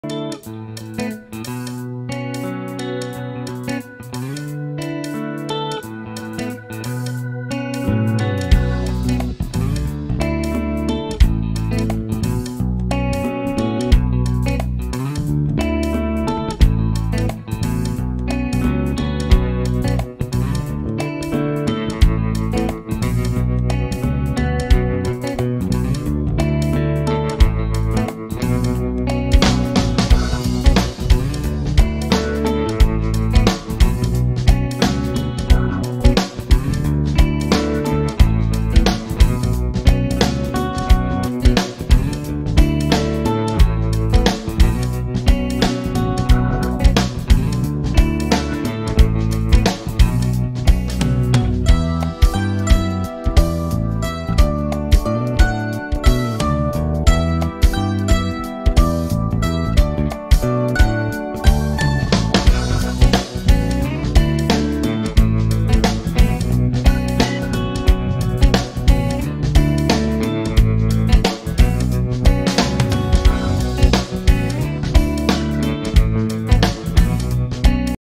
Cos'è per voi l'amore senza fine?
L'amore vero non è quello malato in cui si ama solo con l'intento di possedere,ma ci viene spontaneo e sappiamo prendercene cura ogni giorno.
Adesso tocca a voi scrivermi cosa ne pensate dell'amore e come già sapete tutto quello che scriverete nei commenti andrà sulla mia rubrica virtuale❤

True love is not the sick one in which you love only with the intent of possessing, but it comes spontaneously and we know how to take care of it every day.
Now it's your turn to write me what you think of love and as you already know everything you write in the comments will go to my virtual column ❤

Editing:Shotcut
Link dei libri miei e di Olimpia: